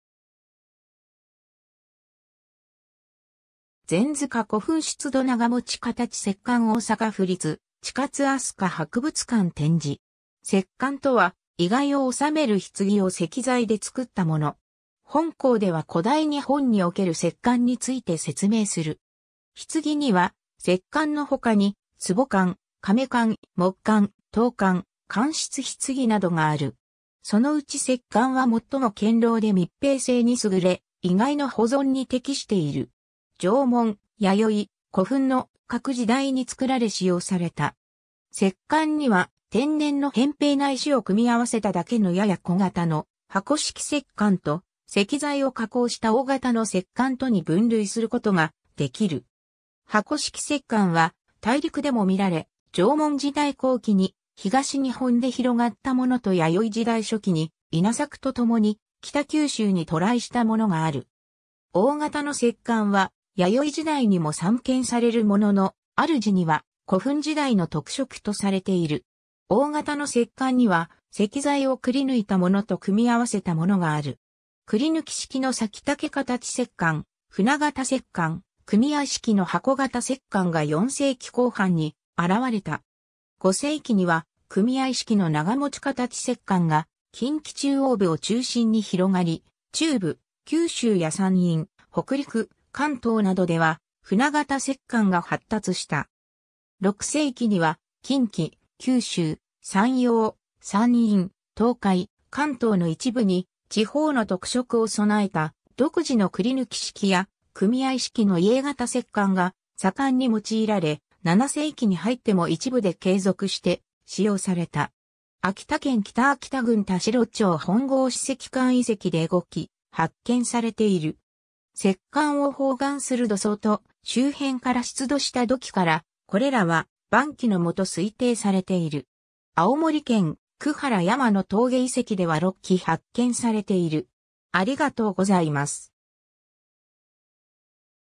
石棺
前塚古墳出土 長持形石棺大阪府立近つ飛鳥博物館展示。
 石棺（せっかん、せきかん）とは、遺骸を収める棺を石材で造ったもの。
本項では古代日本における石棺について説明する。
 棺には、石棺の他に壺棺・甕棺・木棺・陶棺・乾漆棺などがある。
 そのうち石棺は最も堅牢で密閉性に優れ、遺骸の保存に適している。
 縄文・弥生・古墳の各時代に造られ使用された。
 石棺には、天然の扁平な石を組み合わせただけのやや小型の箱式石棺と、石材を加工した大型の石棺とに分類することができる。
 箱式石棺は大陸でも見られ、縄文時代後期に東日本で広がったものと弥生時代初期に稲作とともに北九州に渡来したものがある。
 大型の石棺は弥生時代にも散見されるものの、主には古墳時代の特色とされている。
 大型の石棺には、石材を刳り抜いたものと組み合わせたものがある。
刳抜式の割竹形石棺・舟形石棺、組合式の箱形石棺が4世紀後半に現れた。
 5世紀には組合式の長持形石棺が近畿中央部を中心に広がり、中部九州や山陰・北陸・関東などでは舟形石棺が発達した。
 6世紀には、近畿・九州・山陽・山陰・東海・関東の一部に、地方の特色を備えた独自の刳抜式や組合式の家形石棺が盛んに用いられ、7世紀に入っても一部で継続して使用された。
 秋田県北秋田郡田代町本郷矢石館遺跡で5基発見されている。
石棺を包含する土層と周辺から出土した土器から、これらは晩期のもと推定されている。
青森県久原山野峠遺跡では6基発見されている。